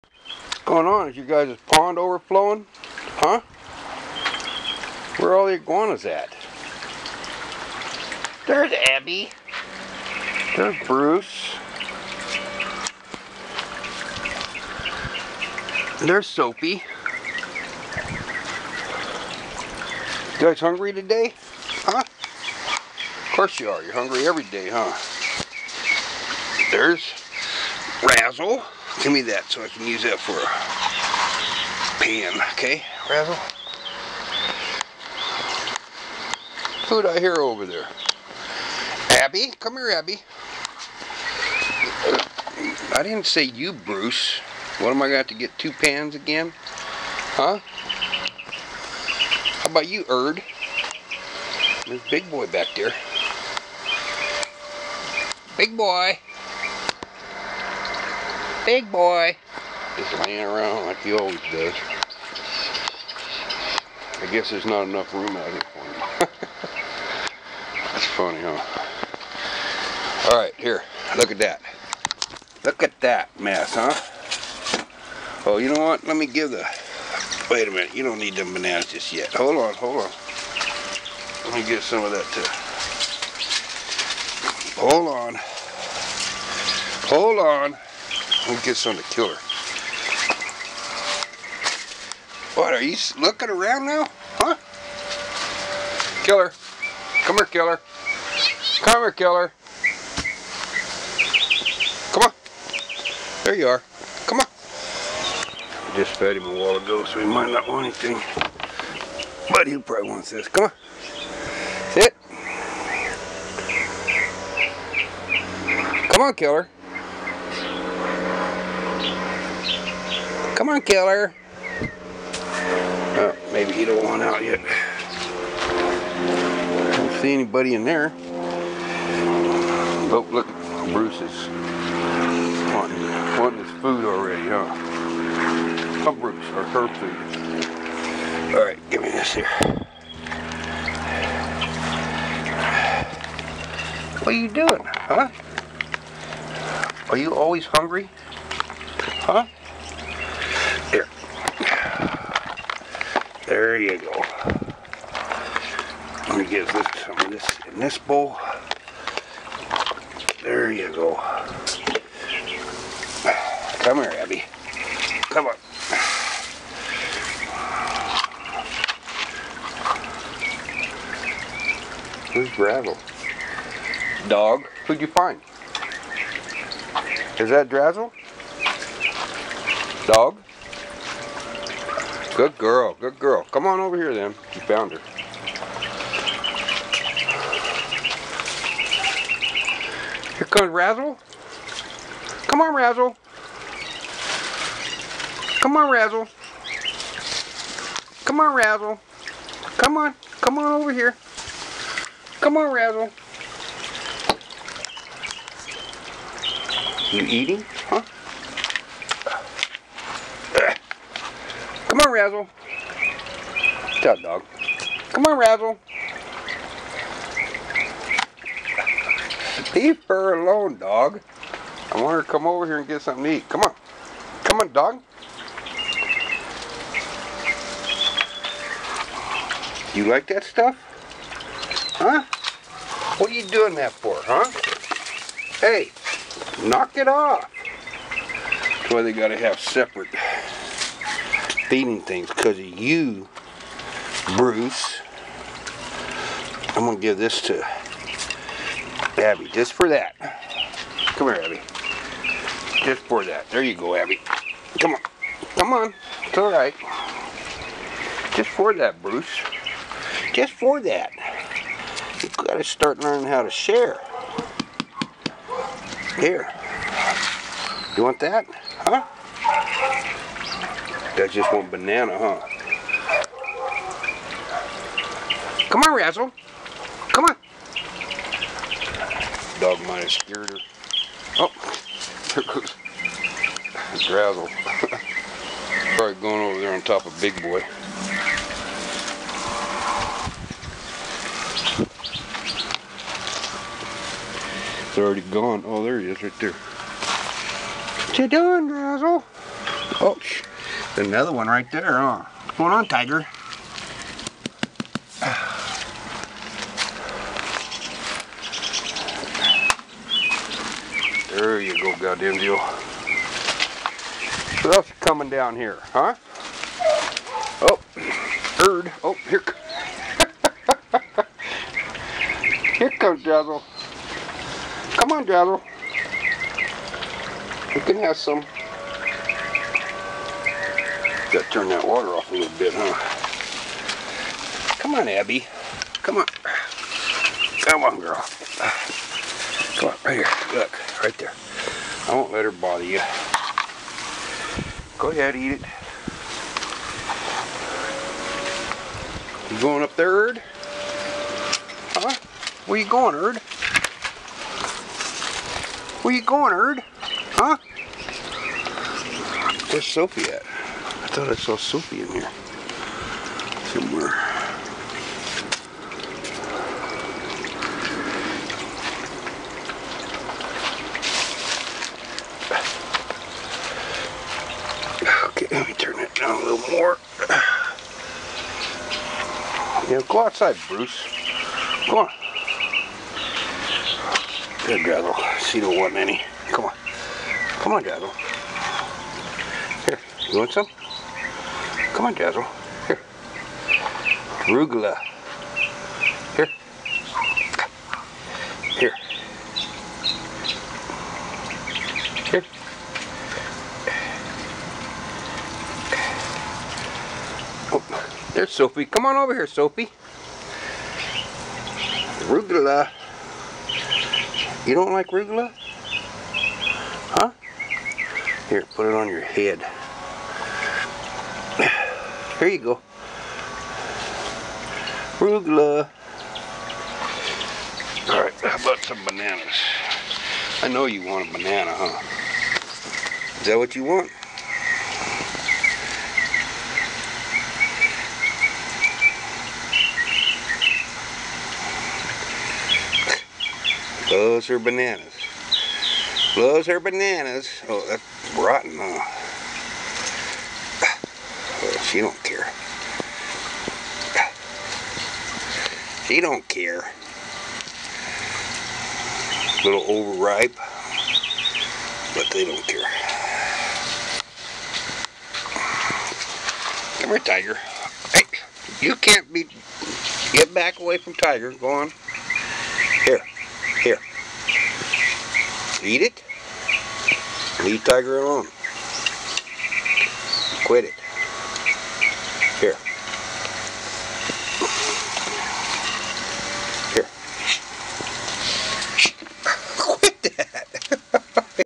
Feeding the Herd,Lizards that is!
Bruce wont let the others eat,until Gaudinzo steps in! I found this video in my channel guide, and it was on private, and I just made it public. This was shot a little  before Bigboy died.